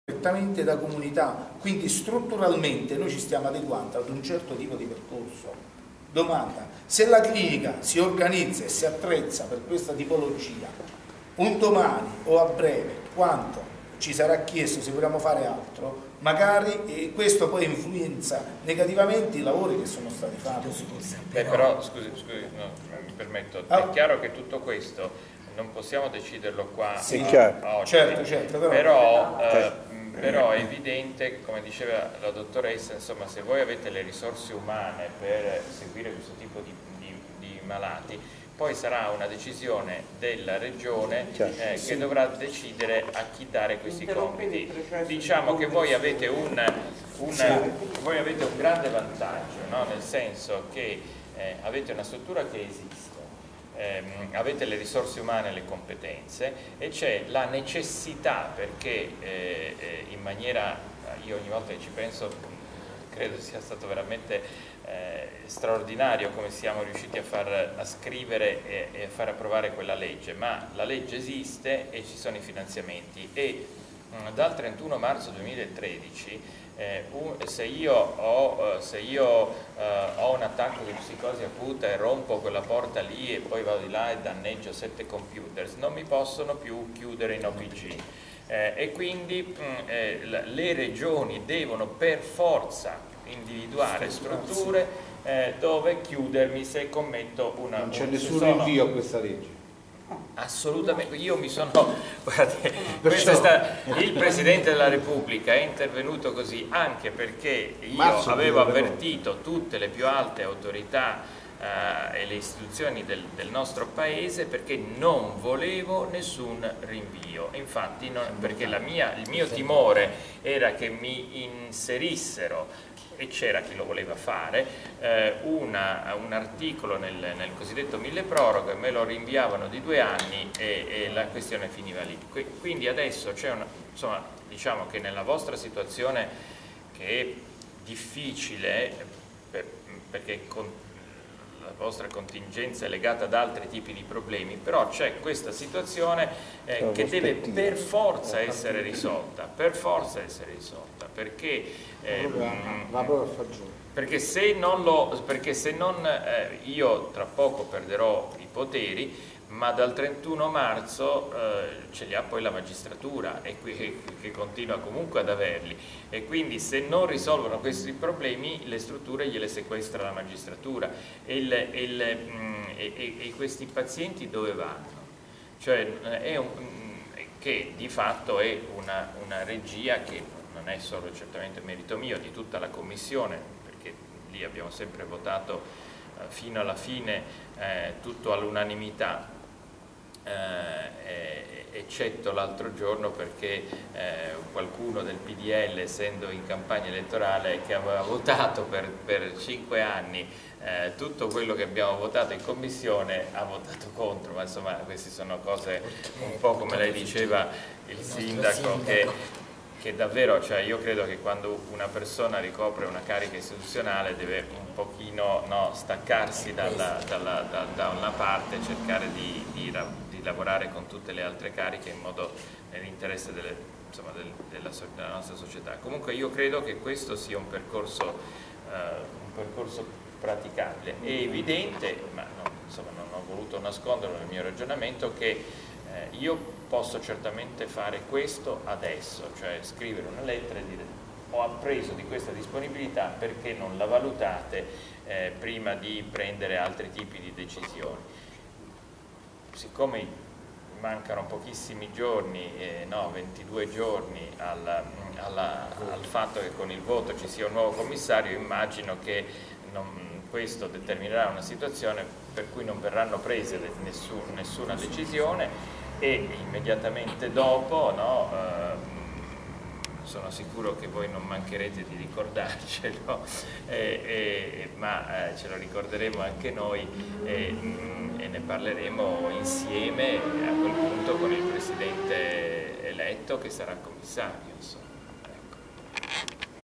Ignazio Marino Formia 3
Formia 2 febbraio 2013, Ignazio Marino, Senatore del Partito Democratico e Presidente della Commissione Nazionale d'Inchiesta per l'efficienza e l'efficacia del Servizio Sanitario Nazionale incontra gli operatori della clinica Sorriso sul Mare